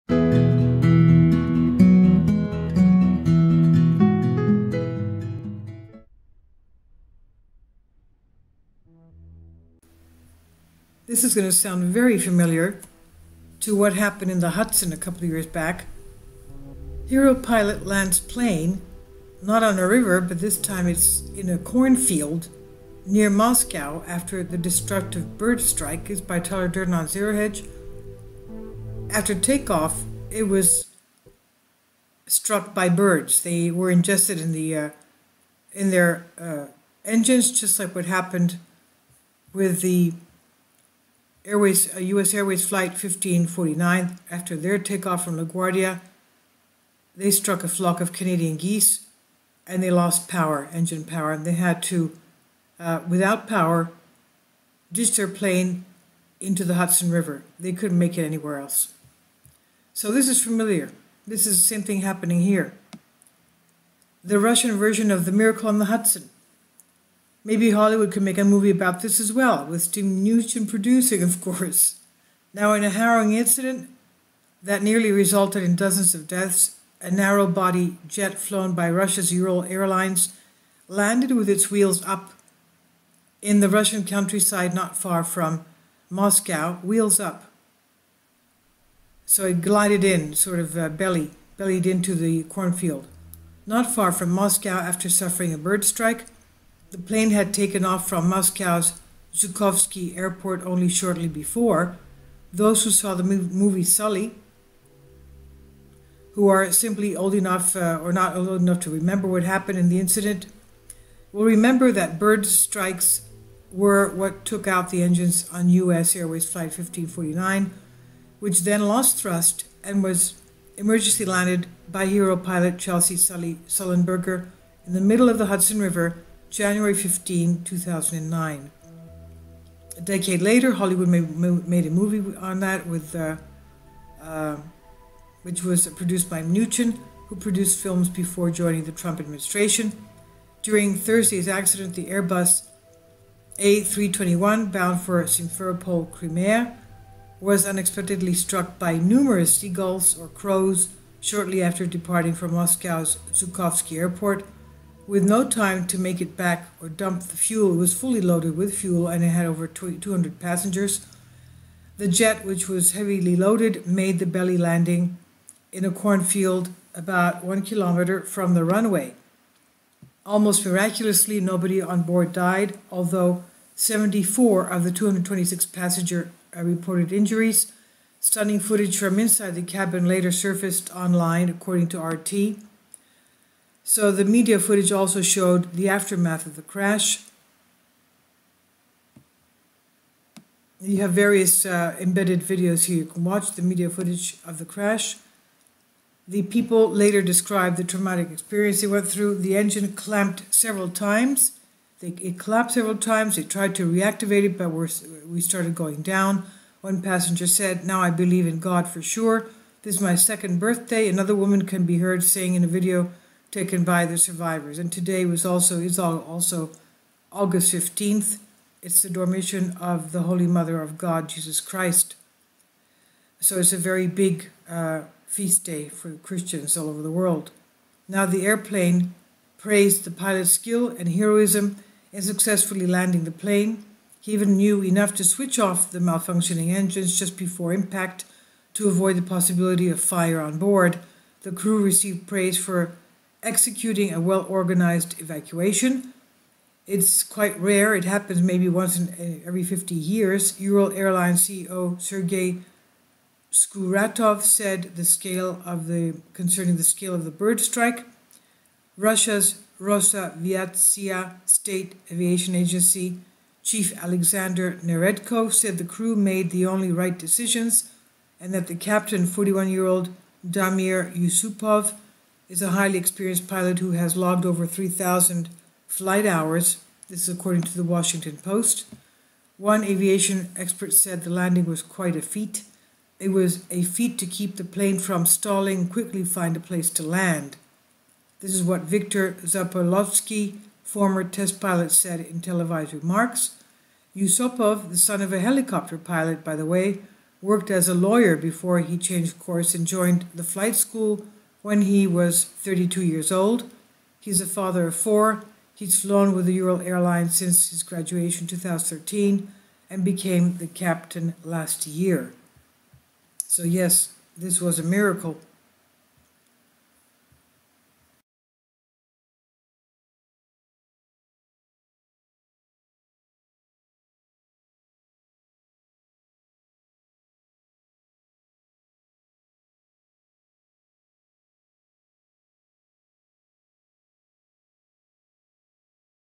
Hero Pilot Lands Plane in CORN FIELD after BIRDS Strike & Stall Engines! All SAVED!
2) Thumbnail image - russian plane lands in corn field ural air.png